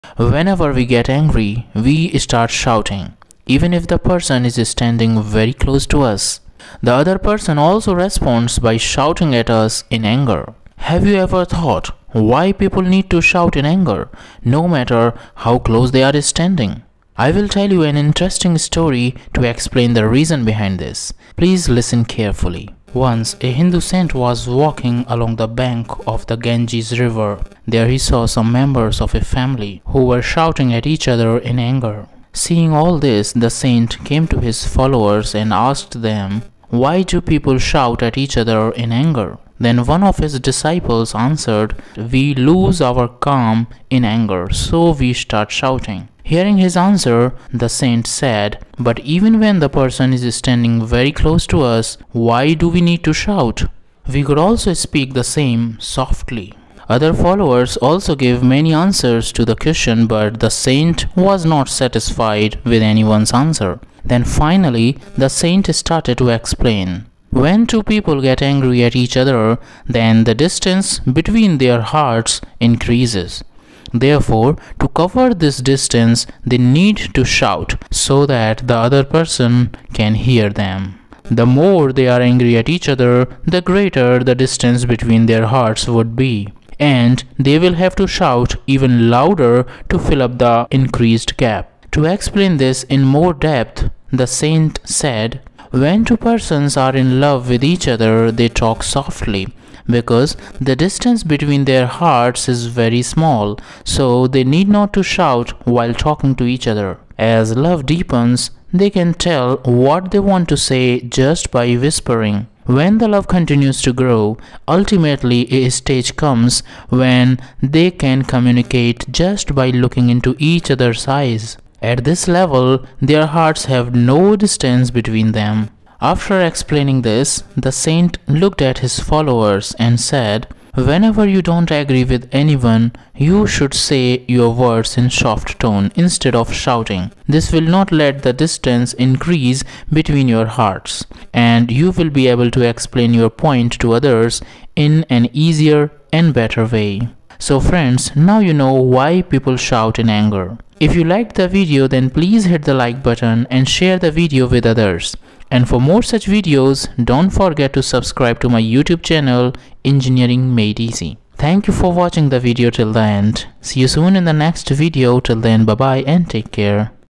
Why Do We Shout in Anger - Why We Shout in Anger - Why We Shout When Angry - Why We Shout
In this video we will learn why do we shout when we are angry and how we can avoid shouting in anger and improve the love, with the help of an inspirational short story. Whenever we get angry, we start shouting even if the person is standing very close to us. The other person also responds by shouting at us in anger.
Have you ever thought why people need to shout in anger, no matter how close they are standing.
I will tell you an interesting short story to explain the reason behind this. Please listen carefully.
Once a Hindu saint was walking along the bank of the Ganges river. There he saw some members of a family, who were shouting at each other in anger.
Seeing all this, the saint came to his followers and asked them, why do people shout at each other in anger? Then one of his disciples answered that we lose our calm in anger, so we start shouting.
Hearing his answer, the saint said, but even when the person is standing very close to us, why do we need to shout. We could also speak the same softly. Other followers also gave many answers to the question but the saint was not satisfied with anyone's answer.
Then finally the saint started to explain-
When two people get angry at each other, then the distance between their hearts increases. Therefore to cover this distance, they need to shout, so that the other person can listen to them.
The more they are angry at each other, the greater the distance between their hearts would be and they will have to shout even louder to fill up the increased gap.
To explain this in more depth, the saint said further-
When two persons are in love with each other, they talk softly because the distance between their hearts is very small so they need not to shout while talking to each other. As love deepens, they can tell what they want to say just by whispering. When the love continues to grow, ultimately a stage comes when they can communicate just by looking into each other's eyes. At this level their hearts have no distance between them.
After explaining this, the saint looked at his followers and said - Whenever you don't agree with anyone, you should say your words in soft tone instead of shouting. This will not let the distance increase between your hearts.  And you will be able to explain your point to others in an easier and better way.
So friends, now you know why people shout in anger.